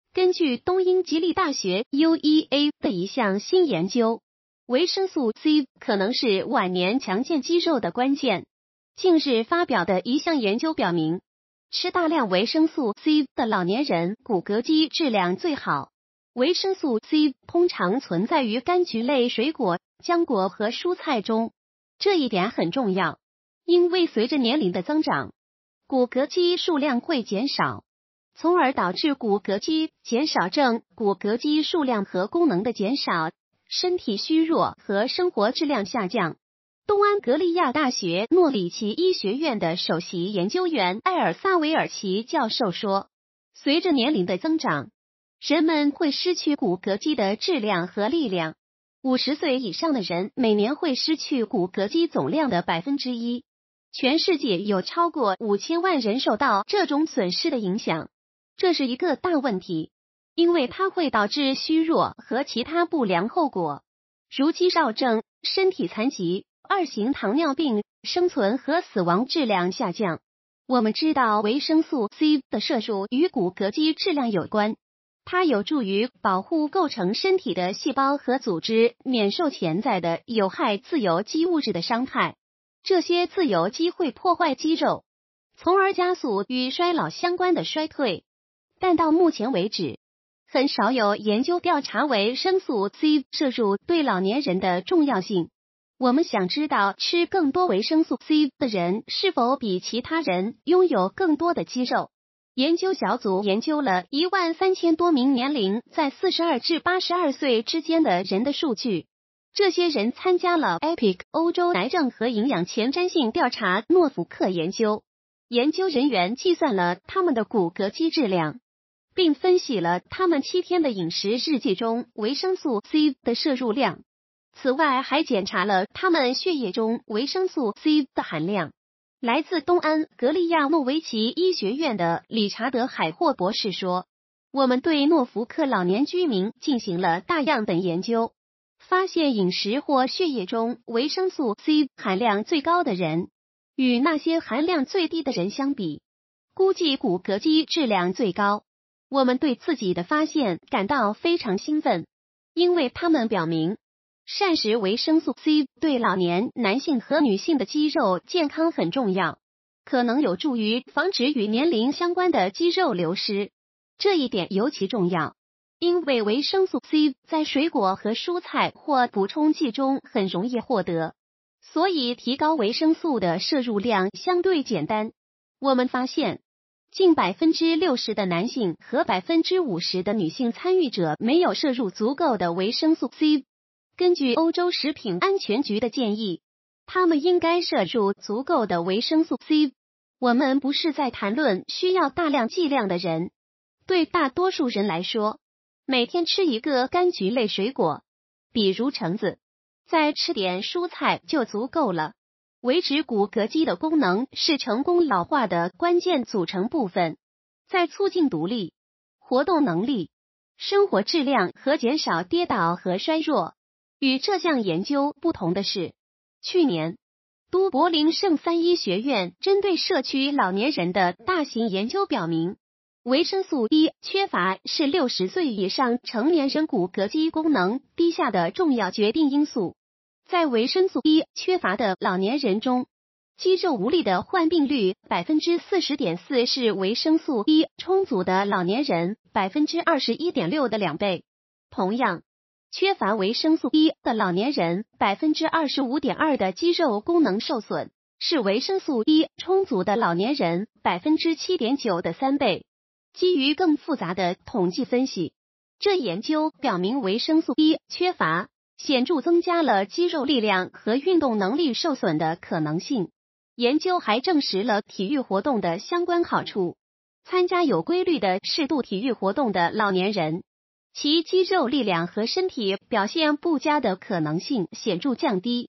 50岁后肌肉强不强健，就看维生素C摄入量是否足够
近日发表的一项研究表明，吃大量维生素C的老年人骨骼肌质量最好，维生素C通常存在于柑橘类水果、浆果和蔬菜中。